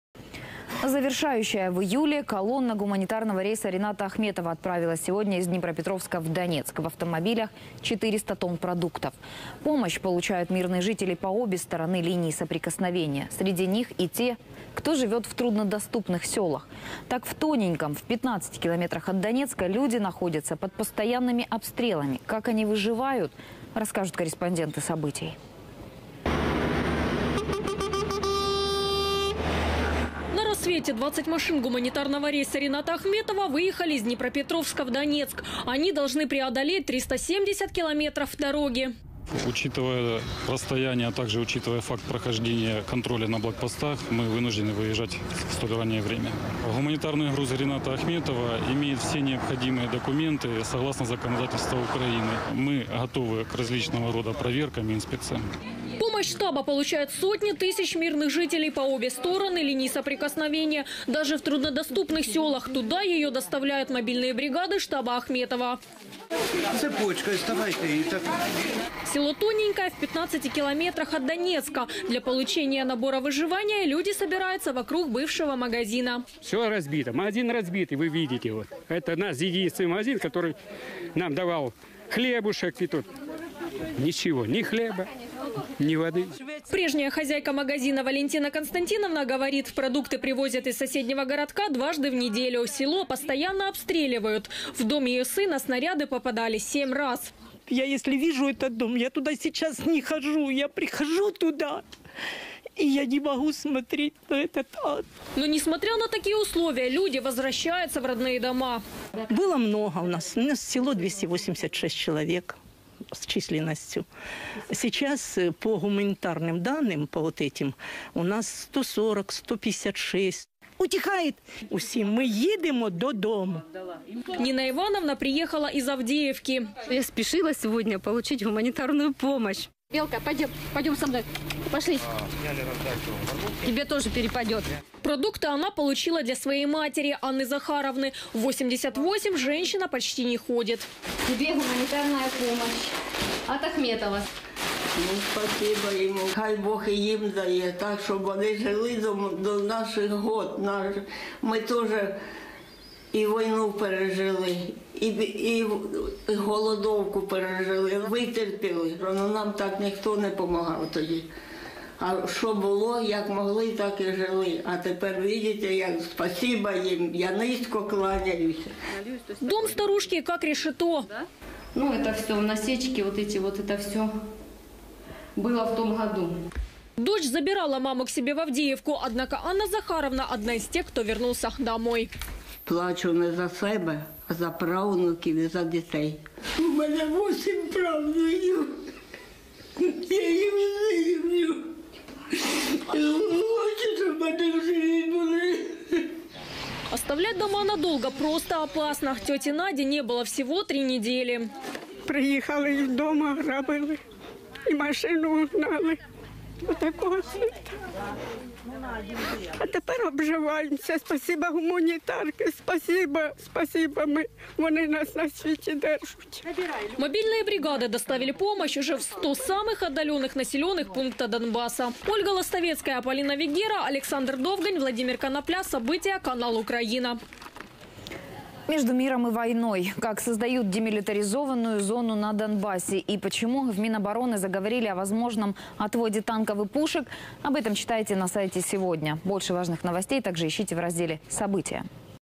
«Плачу не за себе, а за правнуків і за дітей. Хочу, щоб вони живі були»
Село Тоненькое, расположенное на линии соприкосновения, постоянно подвергается обстрелам. В некоторые дома снаряды попадали по семь раз. Численность населения здесь уменьшилась в два раза. Тем не менее жители потихоньку возвращаются домой. Как выживают они? Ведь в селе нет даже магазина, он − разрушен.